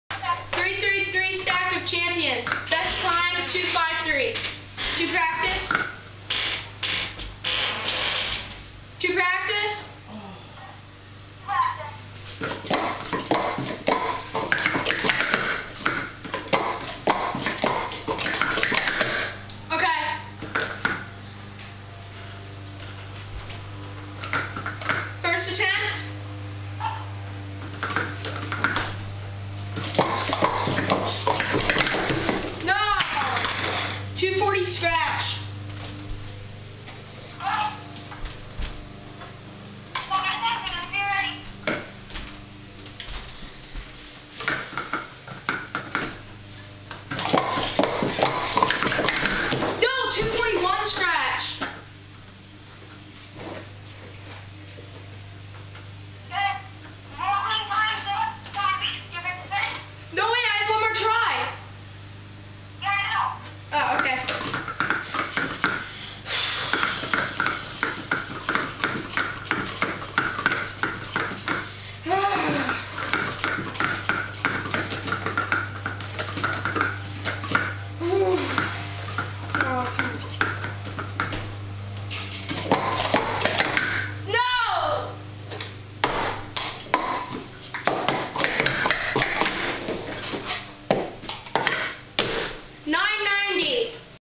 3-3-3 Stack of Champions
this is from the tournament me and joshua had over the phone